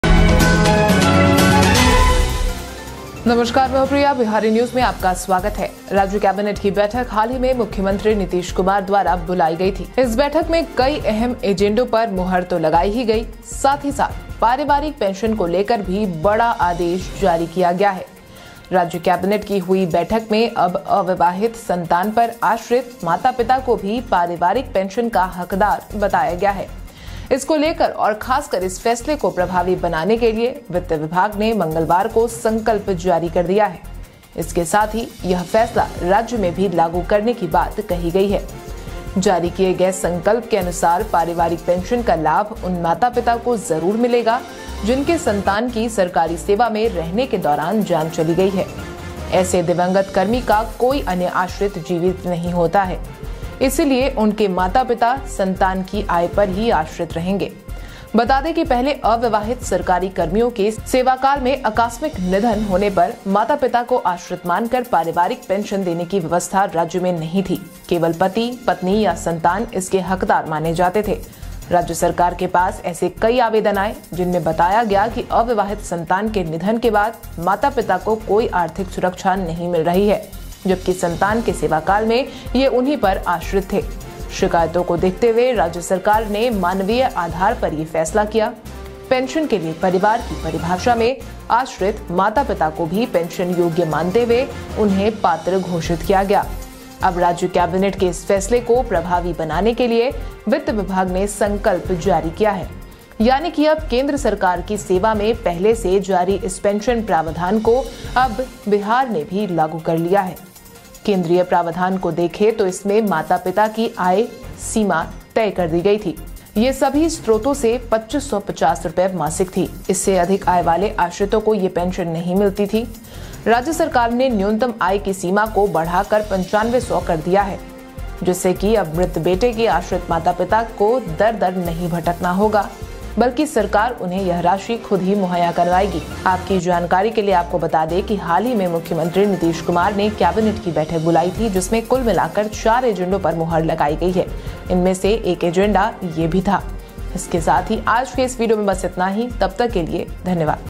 बिहार में अविवाहित संतान पर आश्रित माता-पिता भी होंगे अब पेंशन के हकदार
The state cabinet meeting was recently convened by Chief Minister Nitish Kumar. Parents have also been declared entitled to family pension. Regarding this and especially to make this decision effective, the Finance Department has issued a resolution on Tuesday, along with it has been said to implement this decision in the state as well.